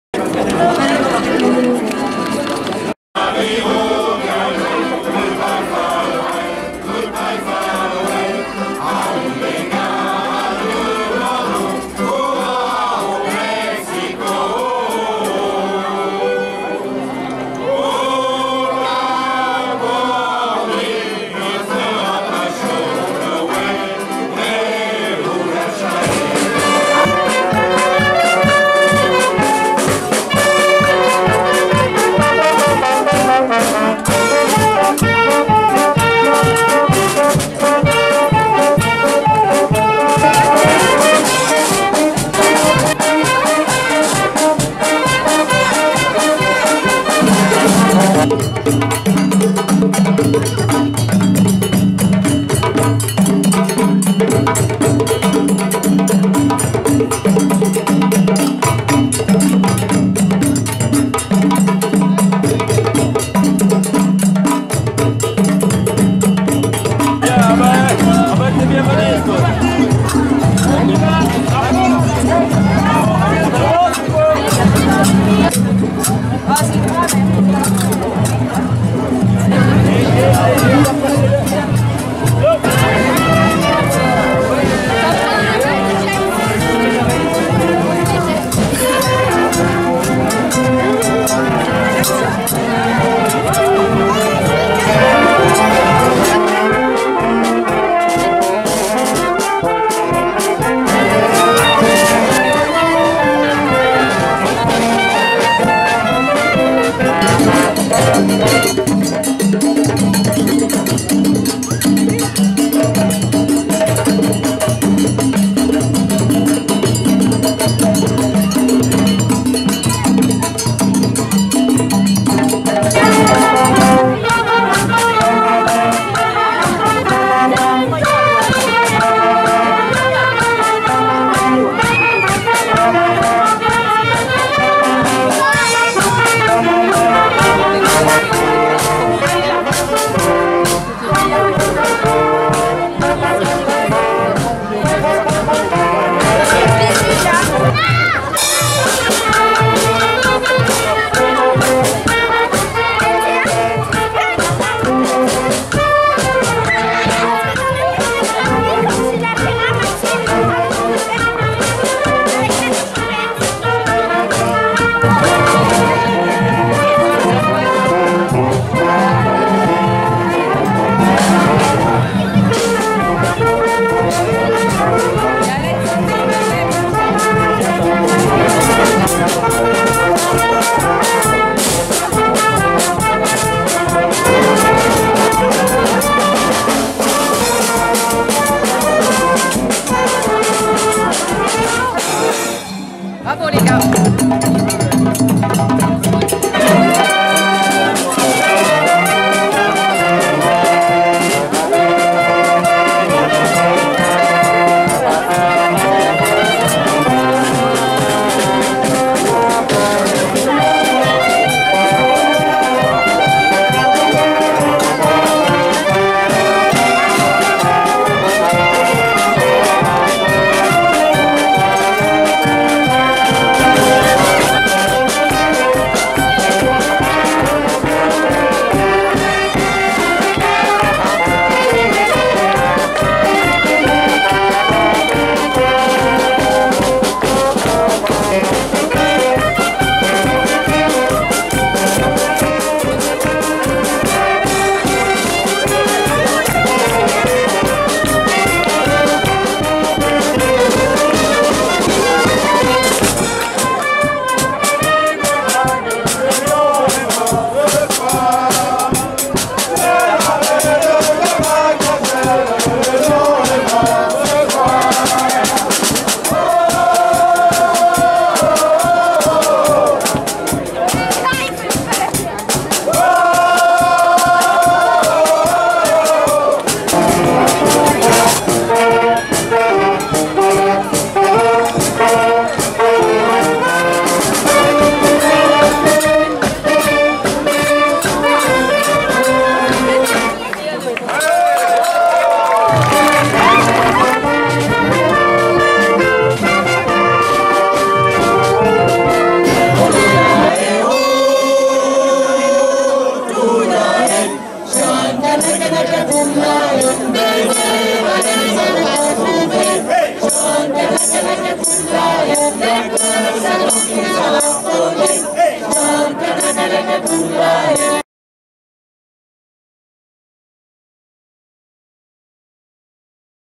carnaval à La Mézière 12 mai 2019
Belle fête que ce carnaval à La Mézière! Un excellent souvenir! Bravo à tous les bénévoles et intervenants.